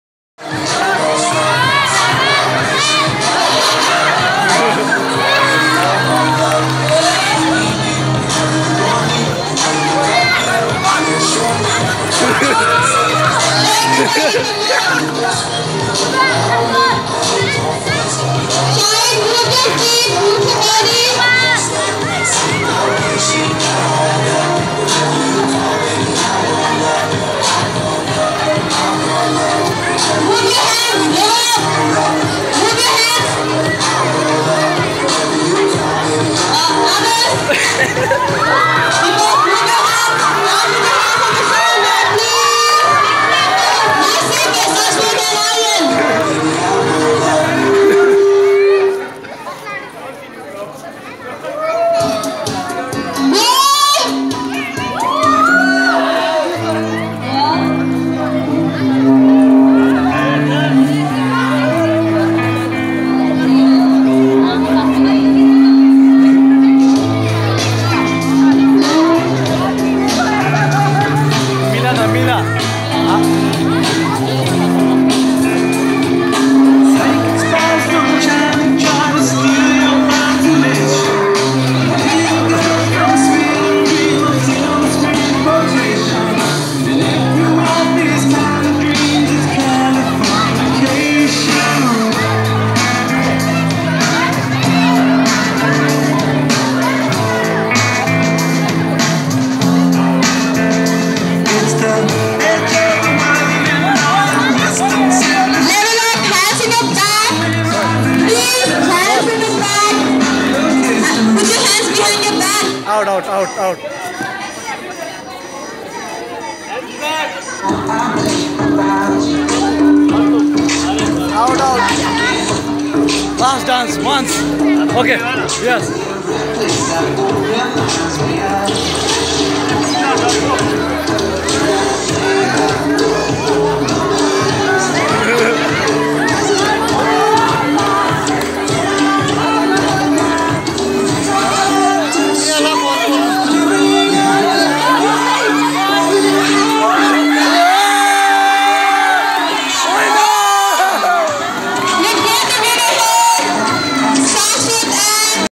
Watch.... how this couple(💃🕴👬) dance with the ballon???((great performance by this couple)
Ballon couple dance 🎈🎈🎈🎉